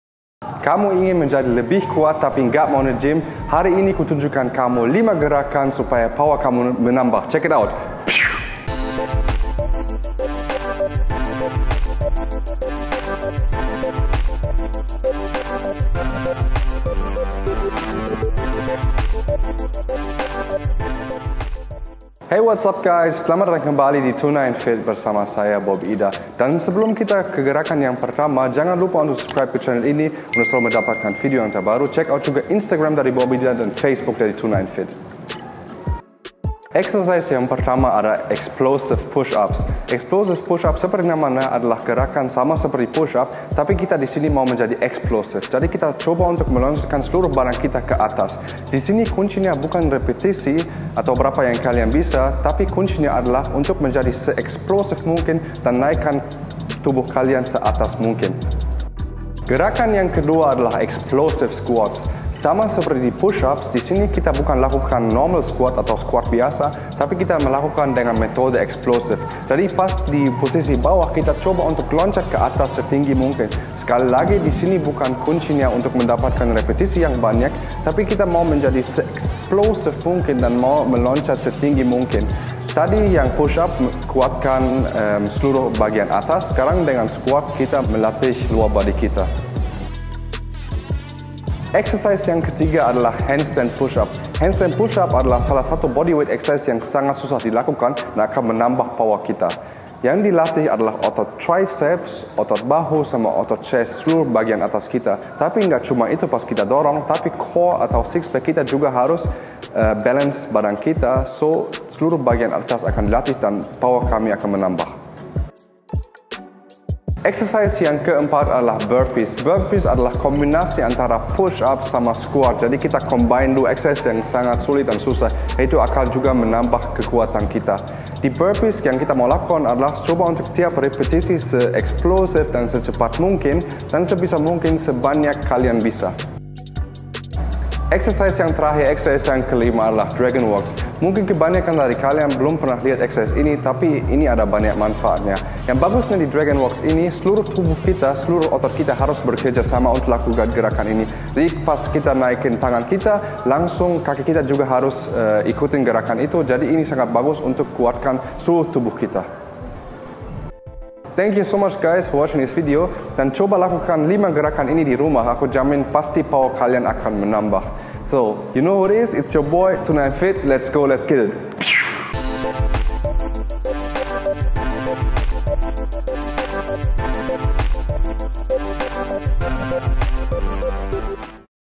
5 LATIHAN UNTUK MENAMBAH POWER DI RUMAH
Hi teman teman fitness,
di video ini aku akan ajarkan kalian gimana caranya menambah power kita di rumah hanya dengan 5 latihan ini.